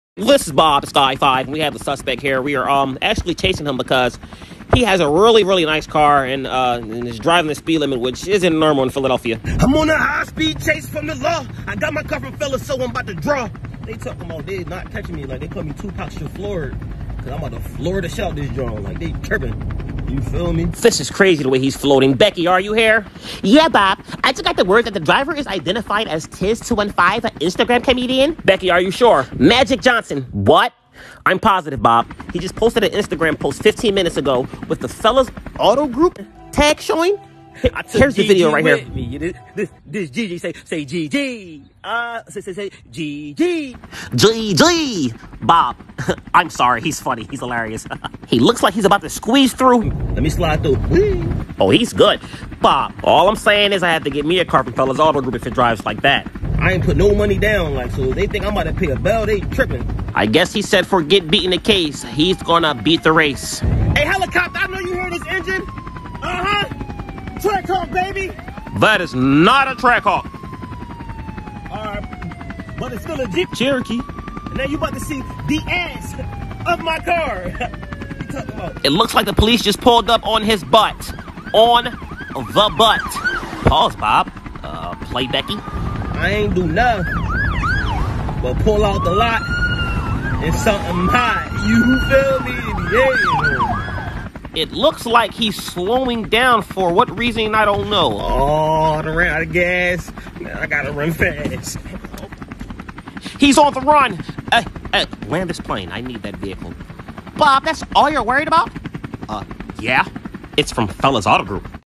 High Speed Chase! Tizz_215
When @Tizz_215 was on a high speed chase the cops was hating on him cuz he got his car from Fellah Auto Group

(NOT a Trackhawk!)

Fast Cars, Jeeps, Trucks, and SUVs all available at The Home of No Money Down